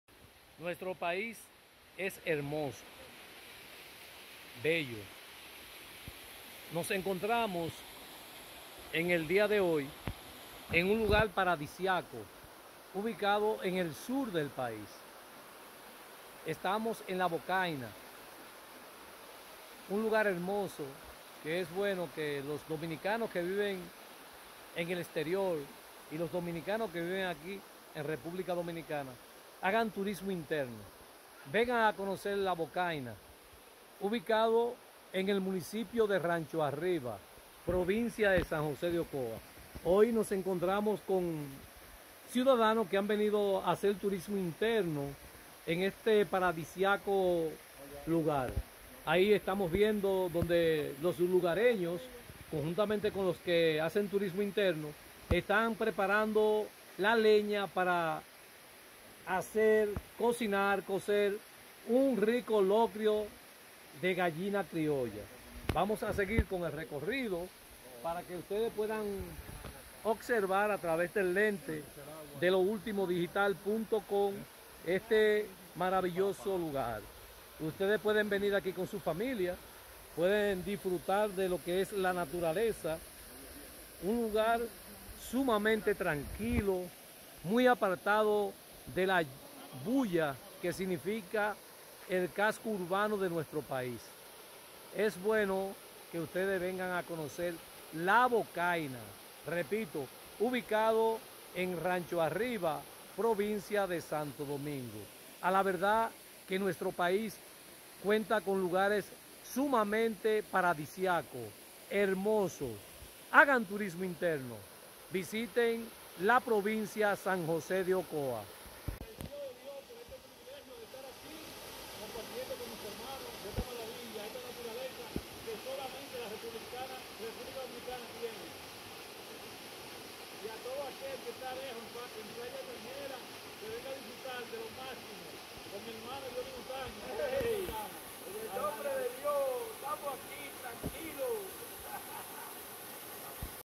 La Bocaina, un paraíso escondido
Loultimodigital.com, es un canal de noticias, deportes, farándula, con programas en vivo  100 x 100 interactivo.
En el corazón de la República Dominicana, entre montañas y valles, se encuentra Ocoa, una tierra cuyo nombre evoca la frescura del agua entre las colinas. Fundada el 28 de diciembre de 1858 por el presidente Pedro Santana, esta región ha florecido con una rica historia y una belleza natural incomparable.